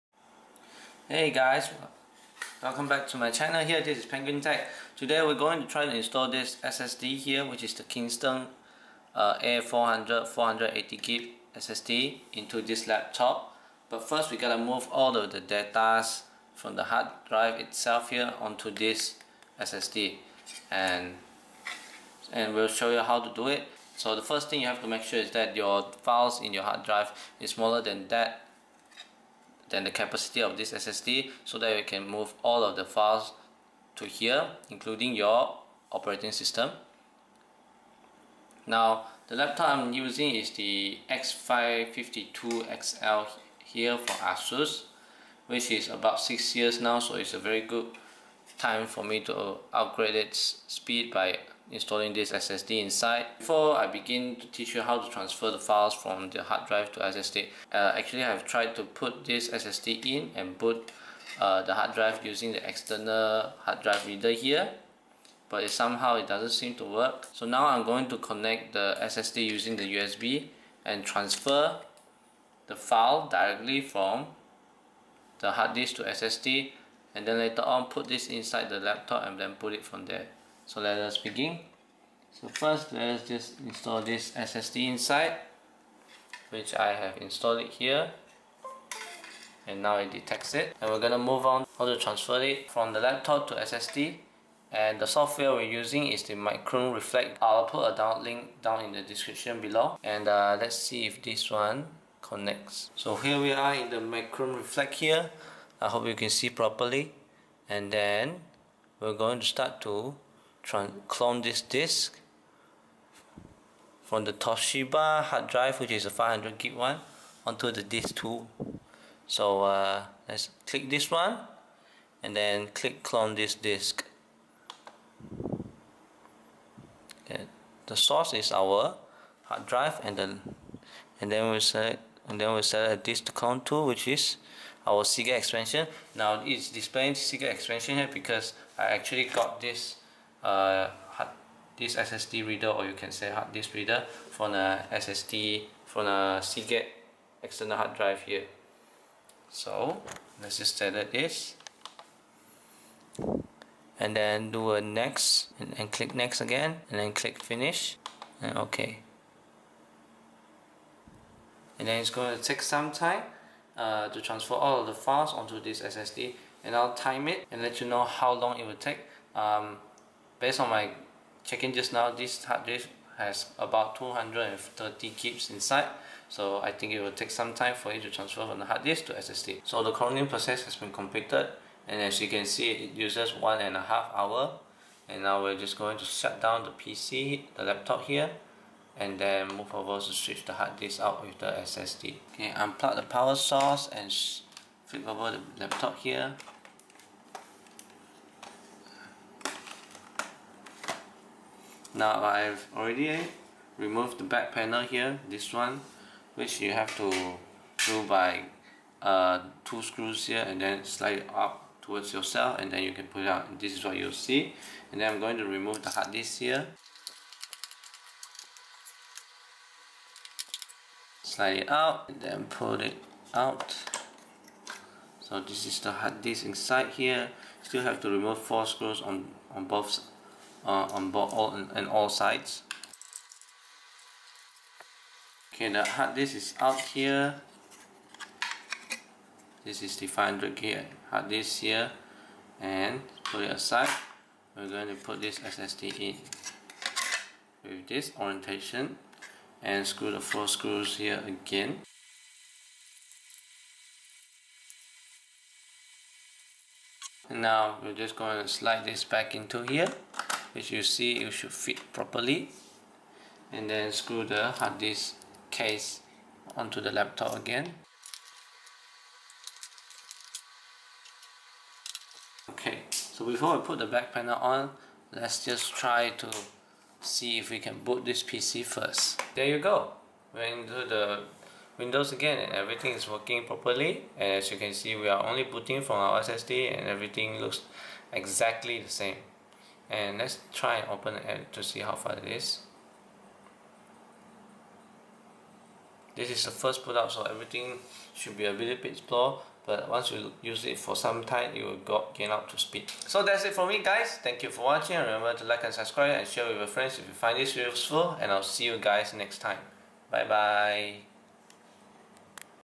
HOW TO INSTALL SSD ON LAPTOP - ASUS X552L
This video will guide you to how to clone your files, including your operating system onto your SSD, followed by installation of SSD into the laptop. Upgrading from HDD to SSD will give a massive boost in terms of system responsiveness, which is extremely useful for older laptops.

The SSD used is Kingston A400 480GB